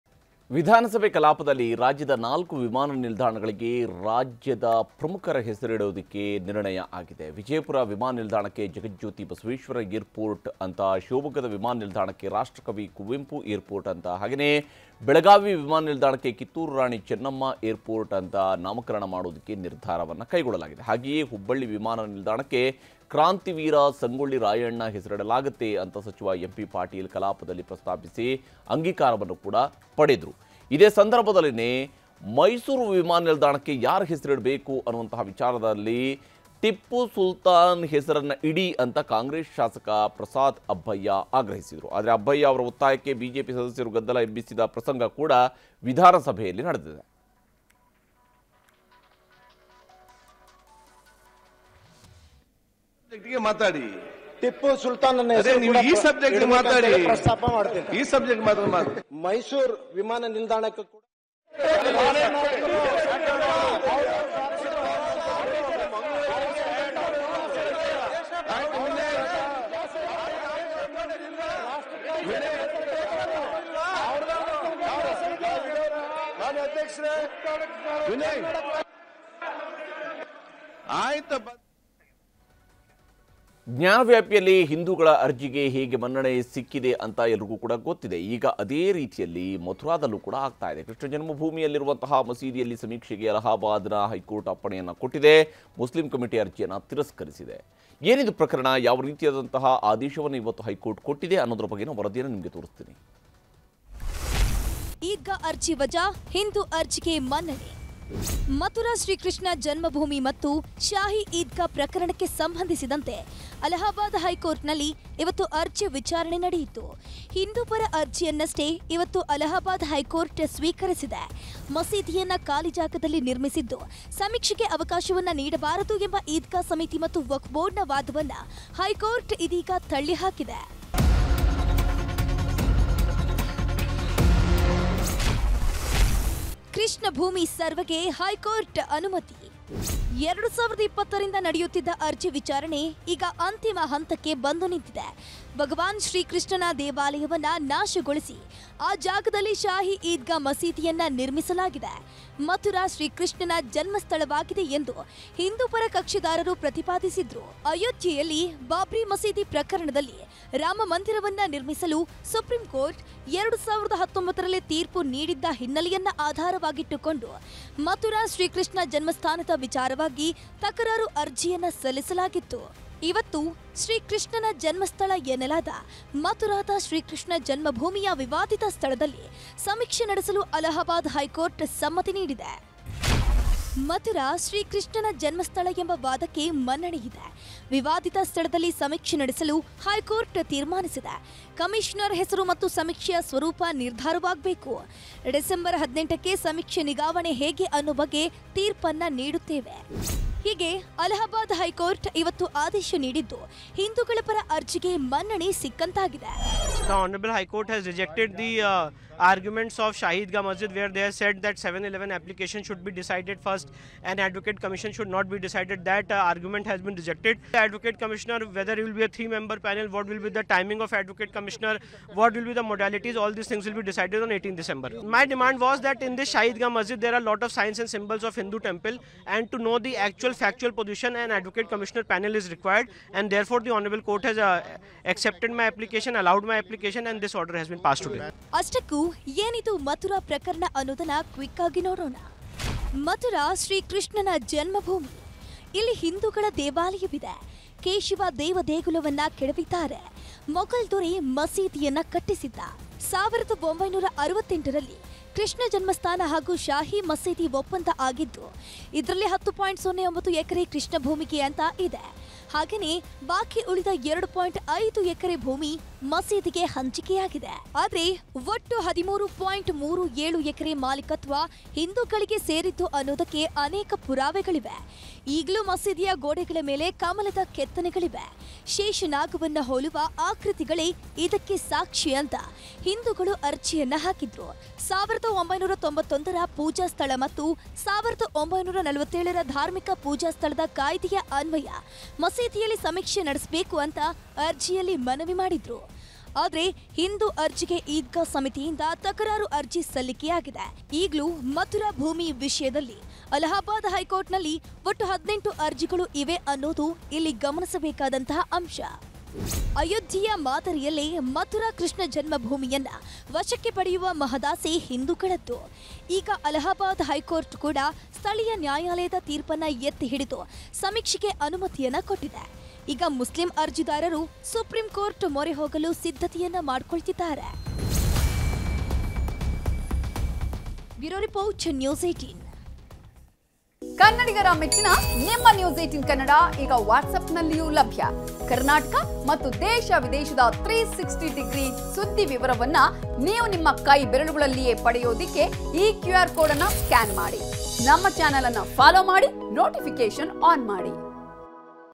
Belagavi Session 2023 | Congress-BJP ಮಧ್ಯೆ ‘ಟಿಪ್ಪು’ ಫೈಟ್​ | Karnataka Assembly Session 2023
News18 Kannada Live | ನ್ಯೂಸ್ 18 ಕನ್ನಡ ಲೈವ್
Karnataka | ಕರ್ನಾಟಕ
Kannada | ಕನ್ನಡ